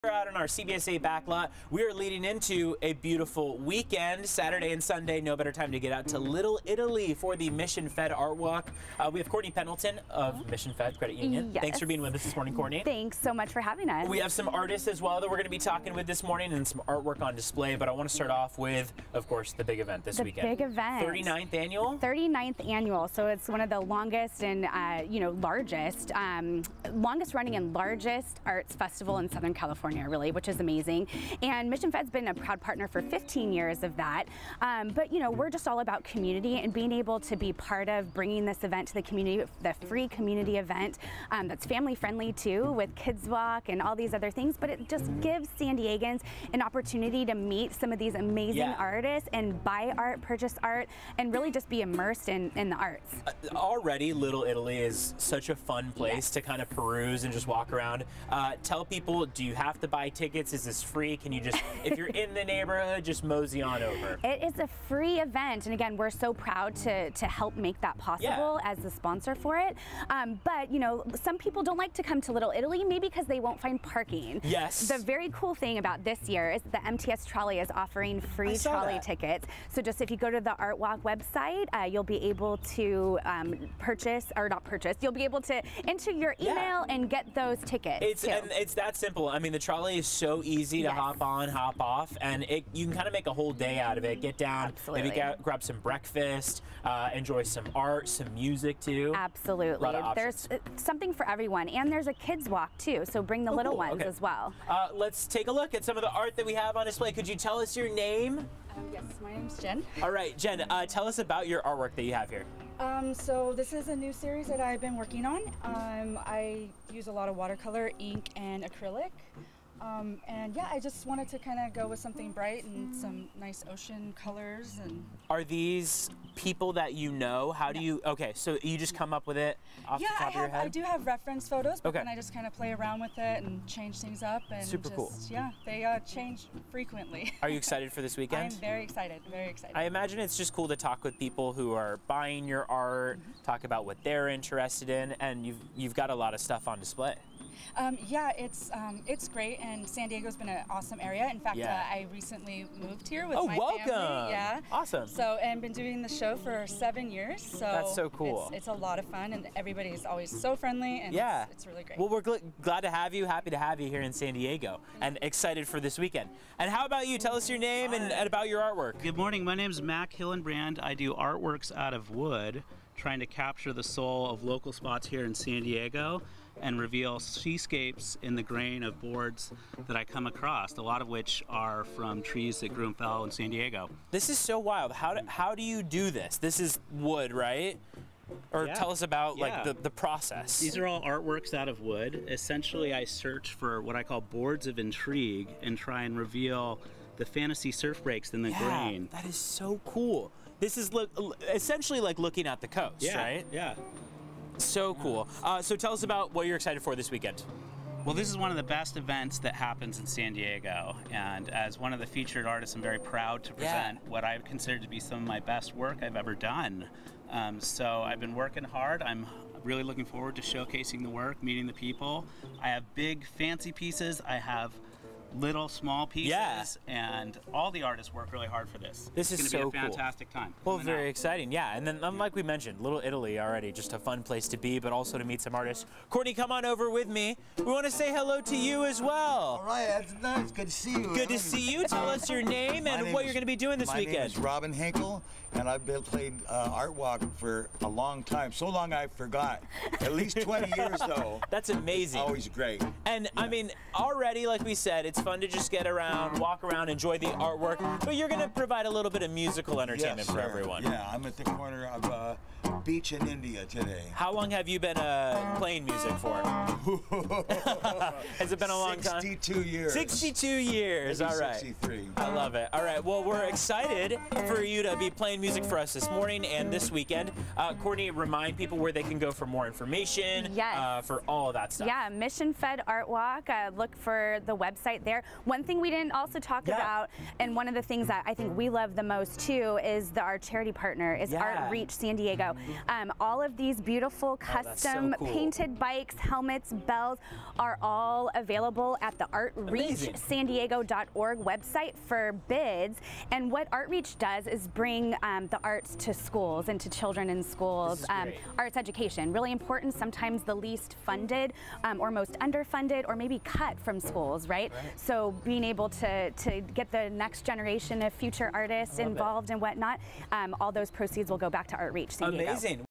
Previewing the Mission Fed ArtWalk in Little Italy
Courtney Pendleton, along with featured artists, talked about this weekend's event.

Mission Fed ArtWalk
Little Italy
Saturday, April 29 - 11:00 a.m. to 6:00 p.m.
Sunday, April 30 - 10:00 a.m. to 5:00 p.m.

·       Mission Fed ArtWalk, now in its 39th year, is the longest running fine art festival in Southern California.
·       The weekend-long event is coming up April 29-30 in Little Italy.
·       The free festival showcases more than 250 local, national, and international artists, where art-lovers can purchase art directly from the creators.
·       The event boasts artists from every medium of art, including painting, sculpture, glass work, photography, fine jewelry and more.
· This year, festival organizers have teamed up with MTS for the first time, offering attendees free Trolley rides to and from the festival.